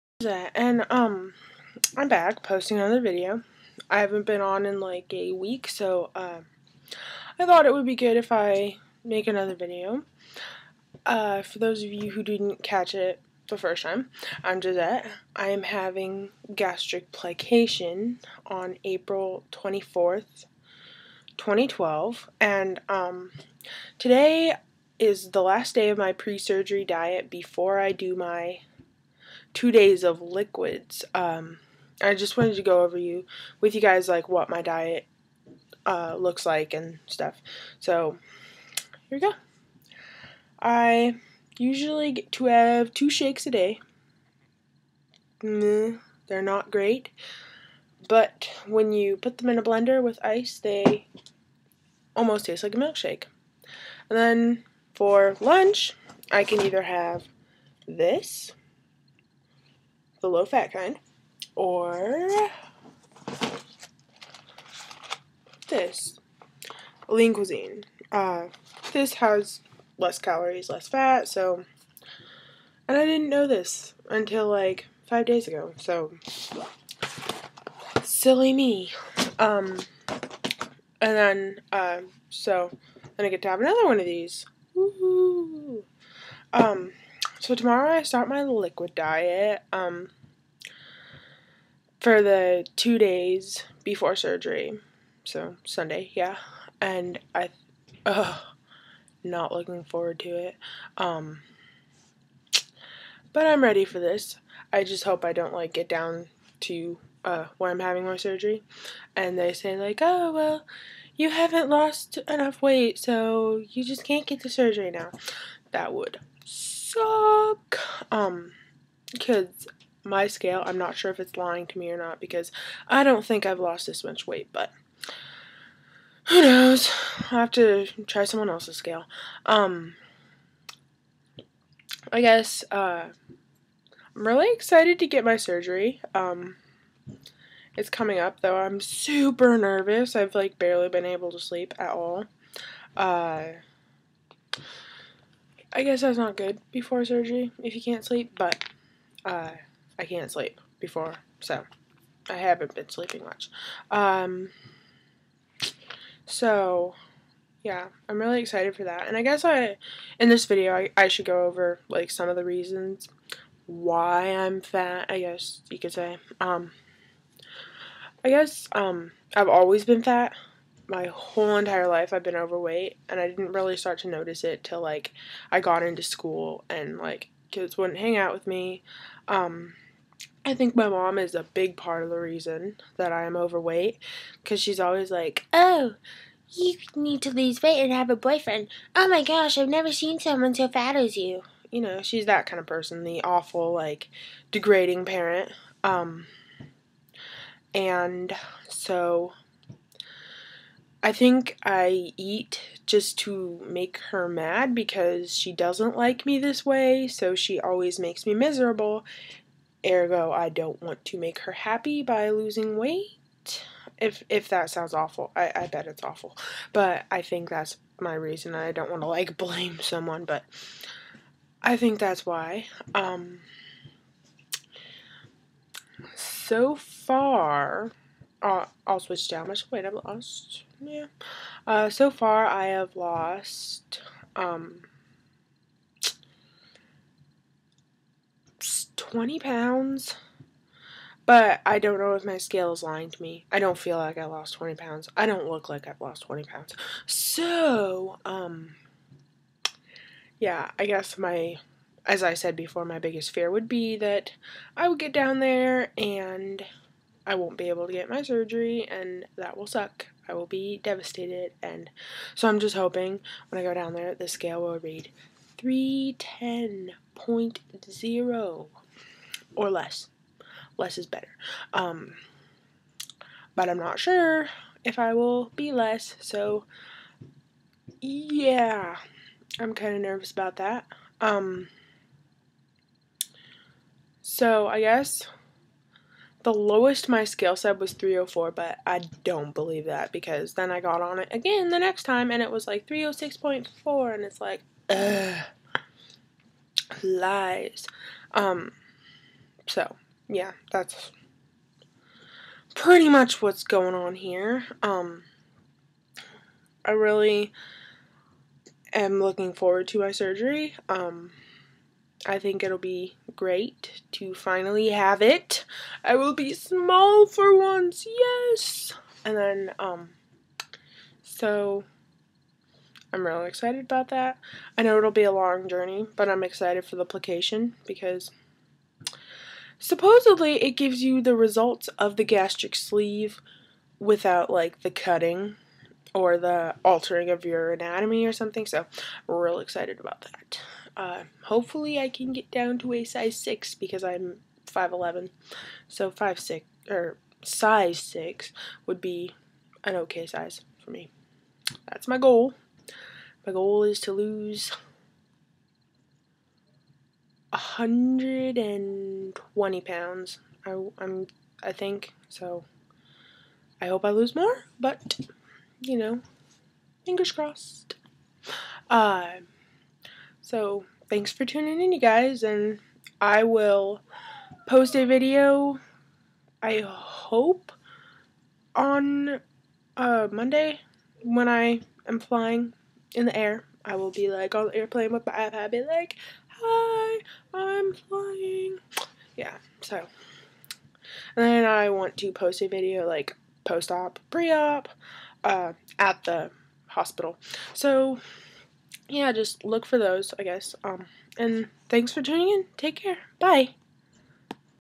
Pre-Op diet & UPDATE!!!
SORRY I LOOK LIKE A HOBO, I DIDN'T STRAIGHTEN MY HAIR
Hi, josette here again! this is my update, sorry i couldn't make it sooner, i had friends visiting from out of state this week and getting everything ready to go down to Mexico has made this week suuuuuuuuuper hectic!!! Thanks for tuning in everyone!!! and since i made this video mt weight has gotten down to 300.4 :D and i started my 2-day pre-op liquid diet today (: